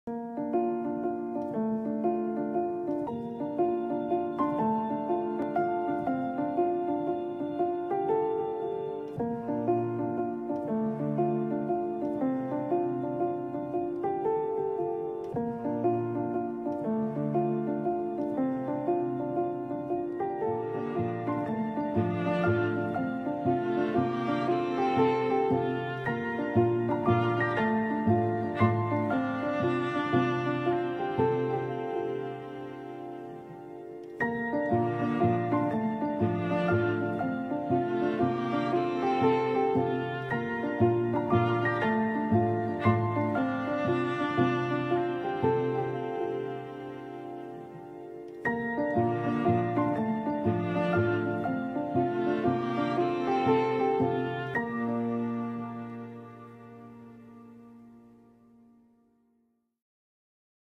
Nature Discoveries 31: Heather-Brutchey-Smith At Marion Tallgrass Trail
We would like to thank Heather-Brutchey-Smith, for capturing some amazing landscape pictures from the Marion Community Foundation Lake, which is a part of Marion Tallgrass Trail.